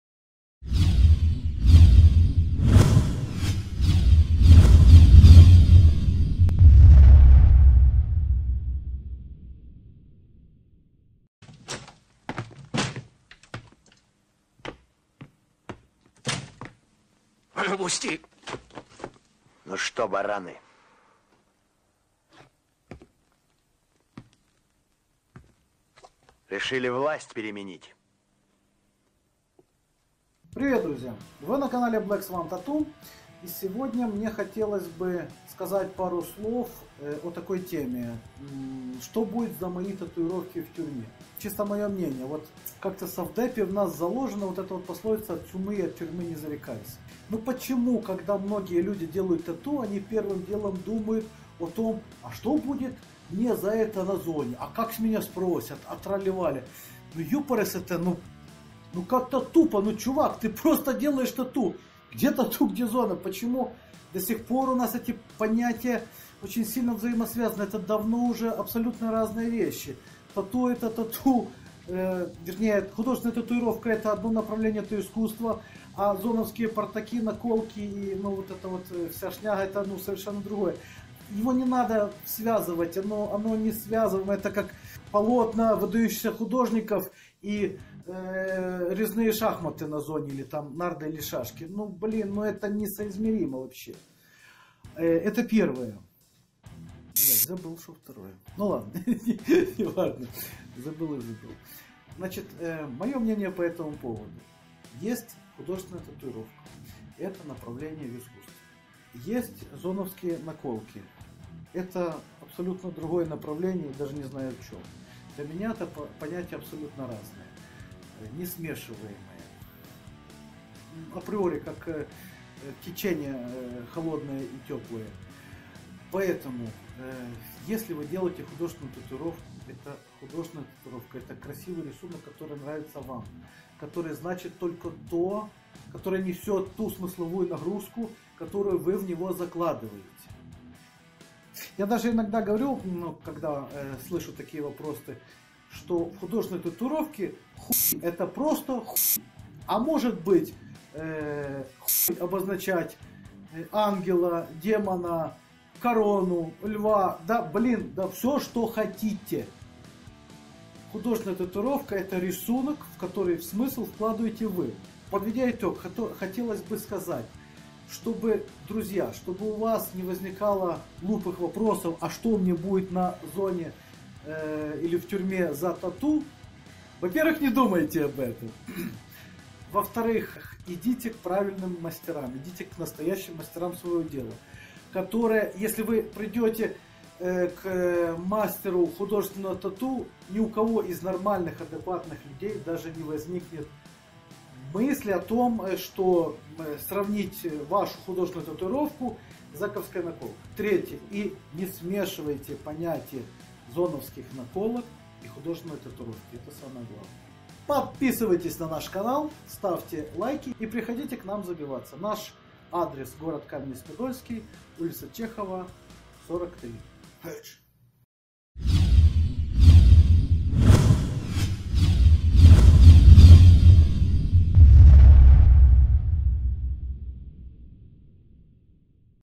Что тебе сделают на зоне за татуировки? | Тюремная наколка и тату
Зоновские наколки и художественные тату - разные вещи, друзья. Не нужно это все путать и переживать, "что мне будет за татуировки в тюрьме?"
Да и в тюрьму тебе, дорогой друг, совсем не обязательно попадать!
В общем, не путайте совсем уже разные вещи, не слушайтесь всех подряд, забивайтесь в тату - студии "Чёрный Лебедь".